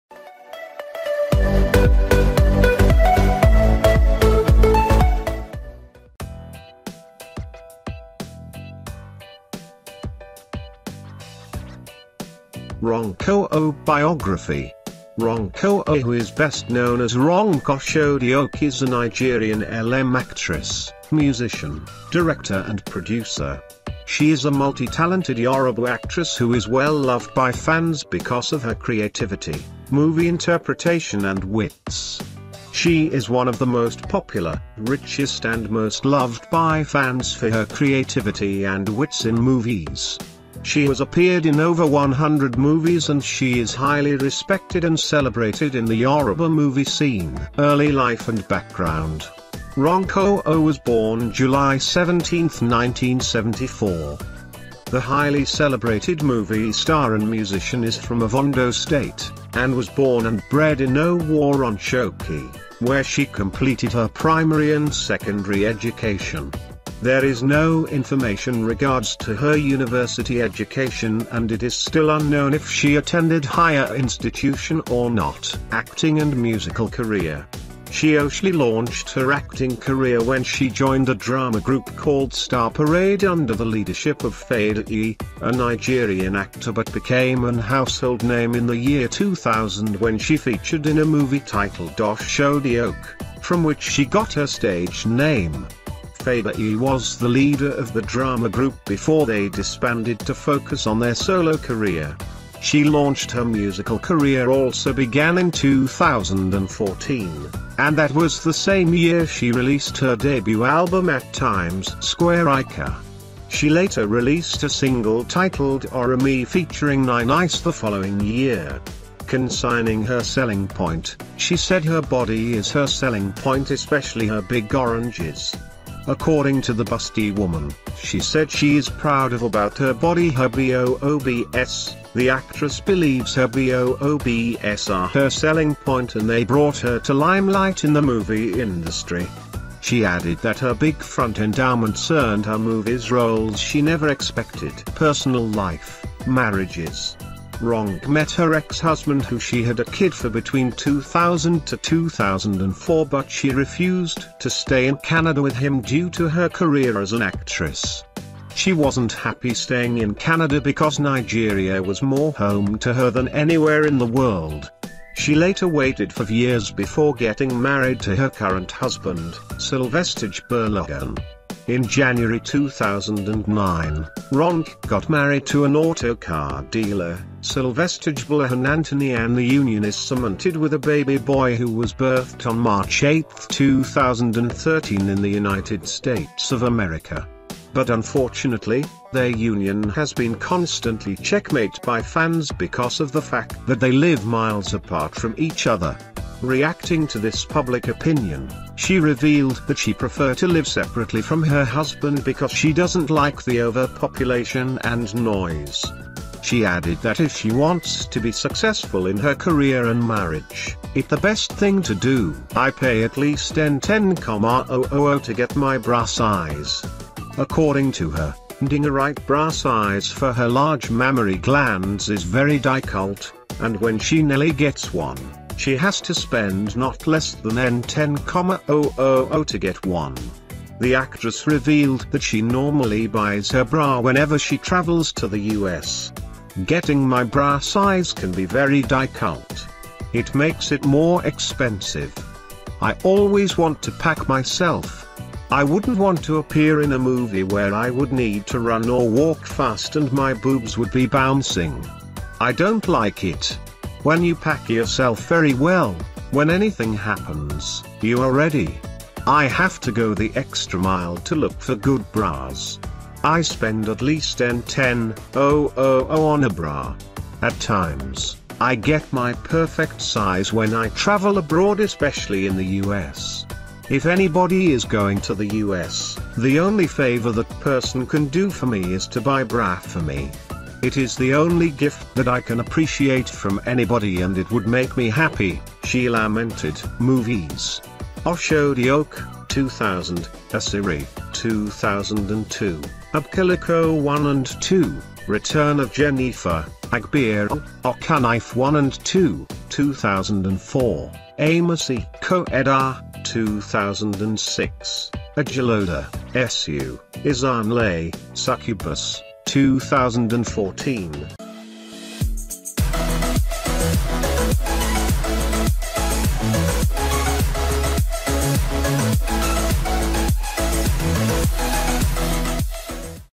Ronke Ojo Biography – Age, Net Worth, Movies, Husband, Children
Ronke Ojo Biography – Age, Net Worth, Movies, Husband, Children - Ronke Ojo who is best known as Ronke Oshodi Oke is a Nigerian lm actress, musician, director and producer. She is a multi talented Yoruba actress who is well loved by fans because of her creativity, movie interpretation and wits. She is one of the most popular , richest and most loved by fans for her creativity and wits in movies. She has appeared in over 100 movies and she is highly respected and celebrated in the Yoruba movie scene.
📽 Robert Mugabe at the un 2018